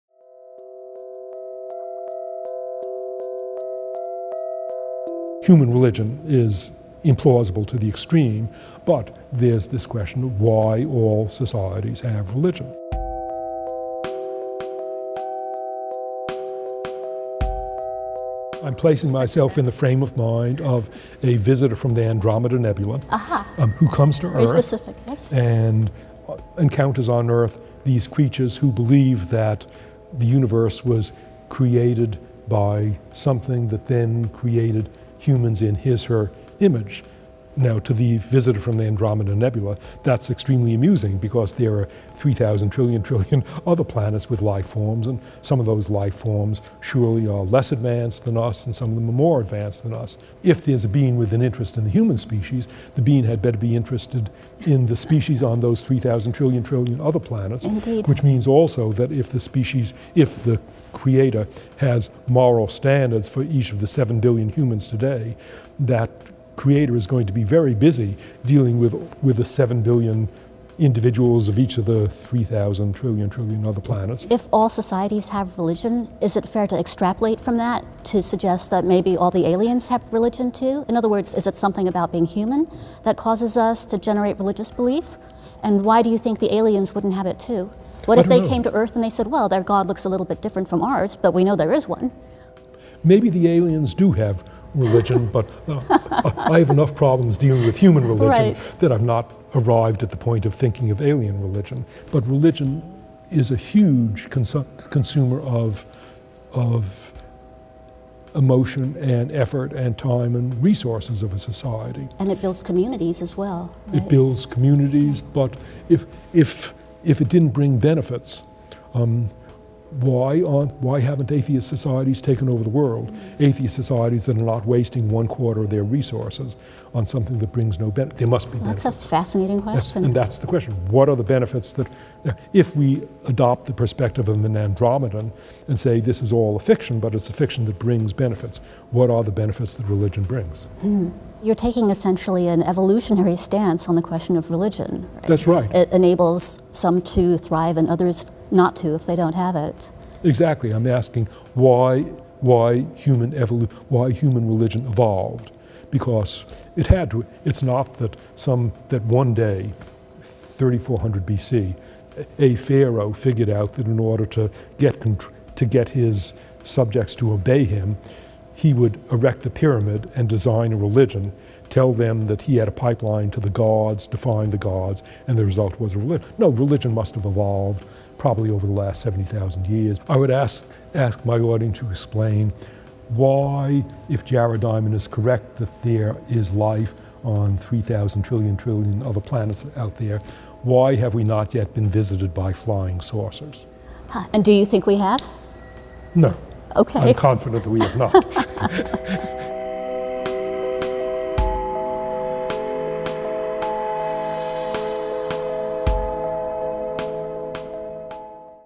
Jared Diamond and Prof. Shadi Bartsch-Zimmer discuss religion
Pulitzer Prize-winning author Jared Diamond delivered the inaugural lecture at the Stevanovich Institute on the Formation of Knowledge on April 20 at the University of Chicago. Before his lecture, he spoke with the institute's director, Prof. Shadi Bartsch-Zimmer, about the evolution of religion. (Video by UChicago Creative)